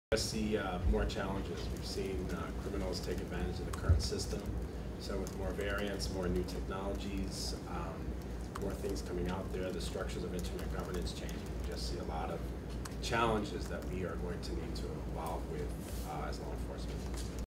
IGF-USA 2012: Bobby Flaim on Internet Security
Bobby Flaim of the FBI spoke on the panel The Changing Landscape of the Domain Name System: New Generic Top Level Domains (gTLDs) and their Implications for Users at IGF-USA 2012, a multistakeholder discussion of Internet issues and the future of communications that took place July 26, 2012, at Georgetown Law Center in Washington, DC. Recorded Imagining the Internet, an initiative of Elon University, North Carolina, USA.